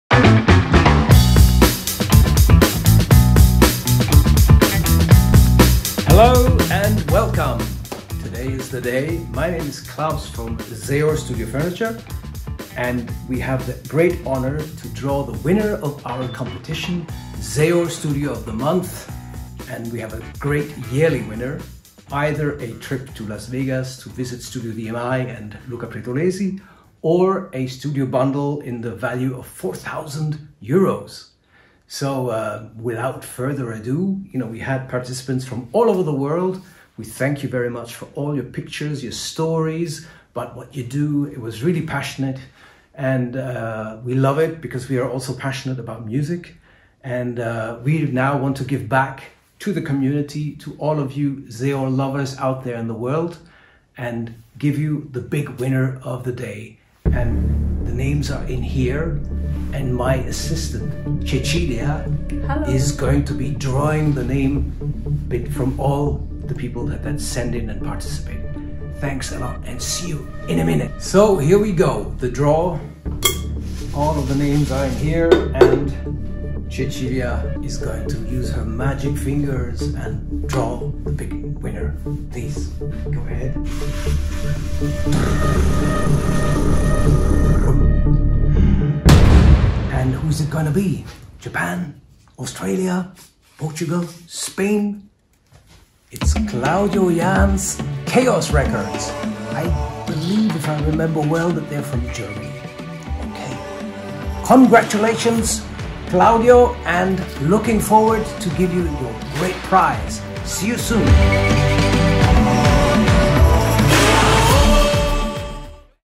Presenting our Zaor Studio Of The Month BIG WINNER: Claudio Jans - Crownjam Studios (Germany)
Congrats Claudio Jans - Crownjam Studios (old brand - Chaos Records)
A studio is a magical place of inspiration, a workplace that expresses both professionalism and individuality. The Final Goal of this contest was to gather producers from all over the world and build a community around music, new ideas, and passion for professional or home-built studios powered by Zaor Studio Furniture! Congratulation to all participants!